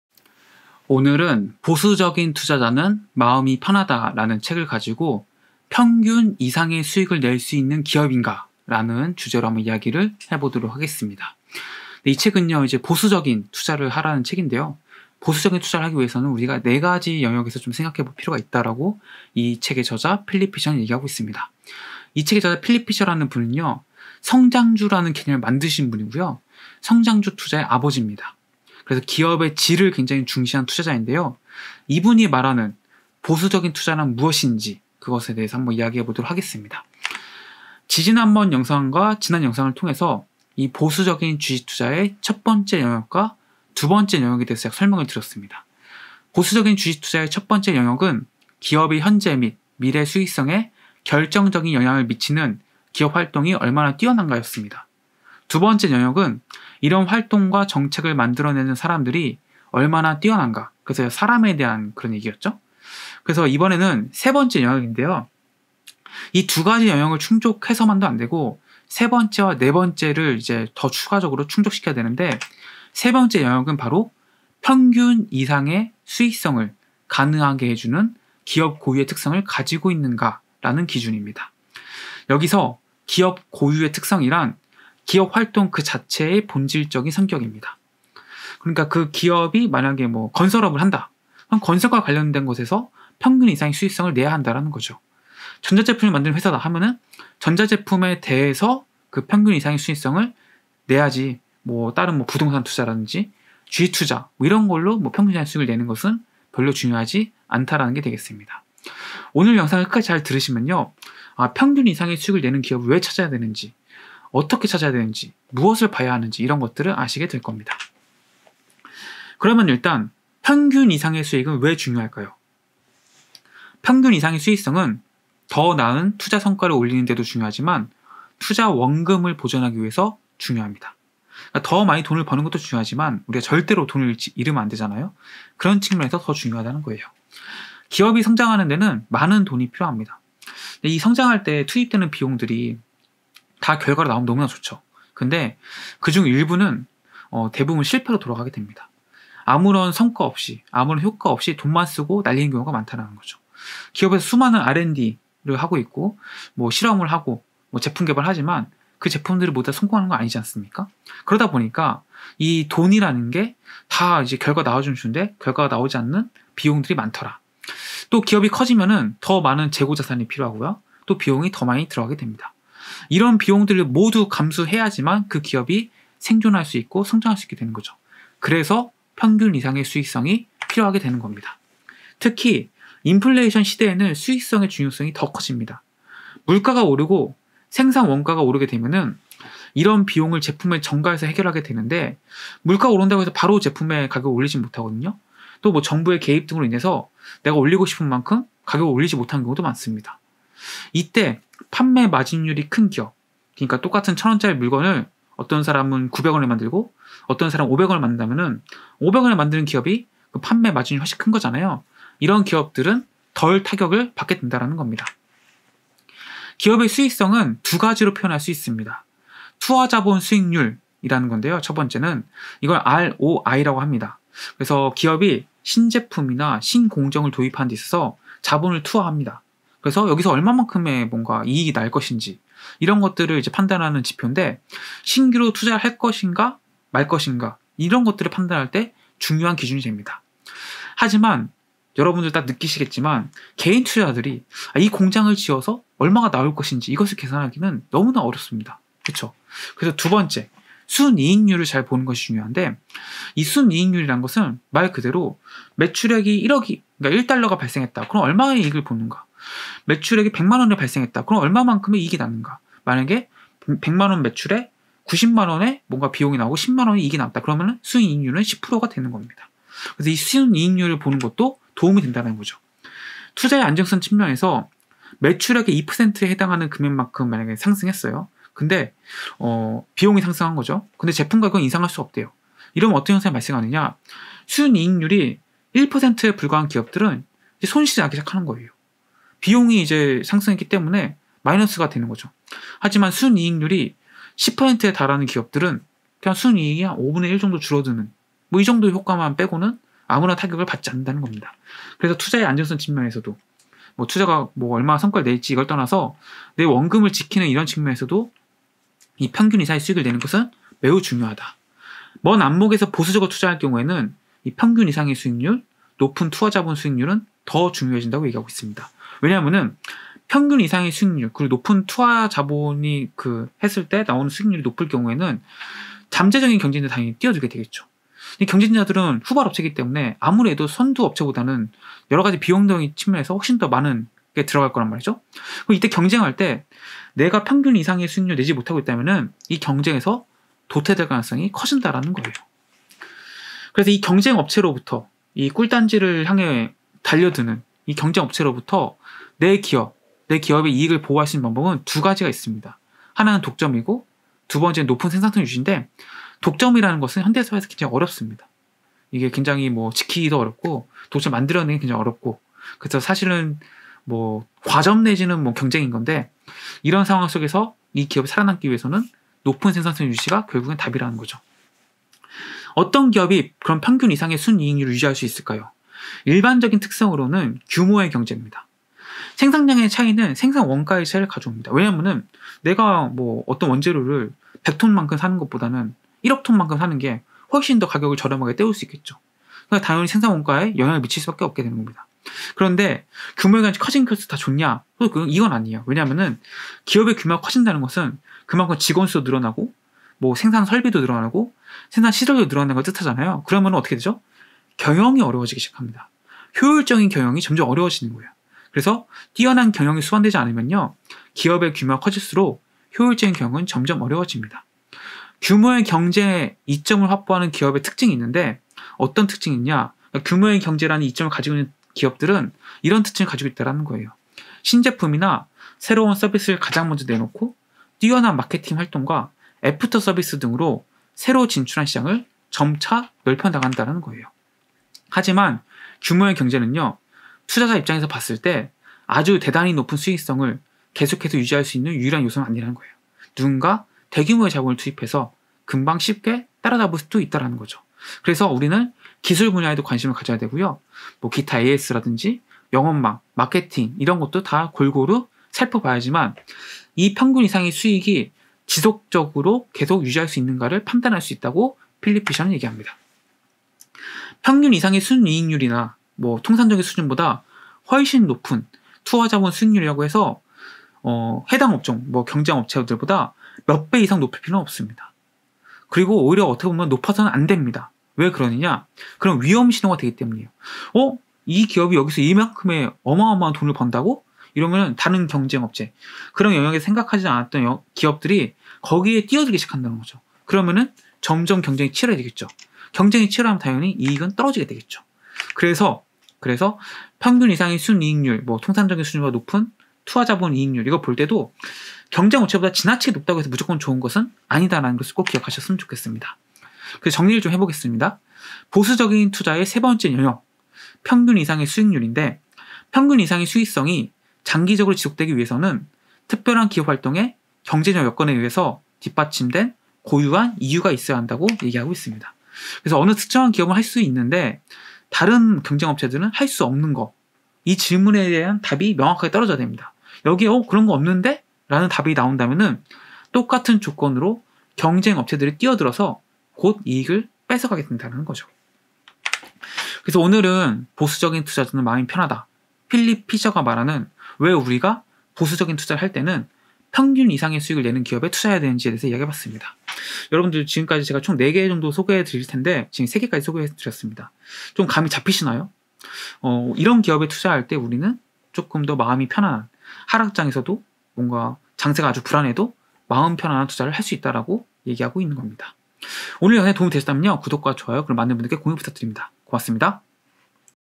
너무 순이익률이 높은 기업은 신중하게 접근해야 하는 이유 I 필립피셔 I 투자고전 I 주식투자 I 보수적인투자자는마음이편하다 3
📚 필립피셔, 보수적인 투자자는 마음이 편하다

자기계발 책을 자세하게 강의하는 책리뷰 채널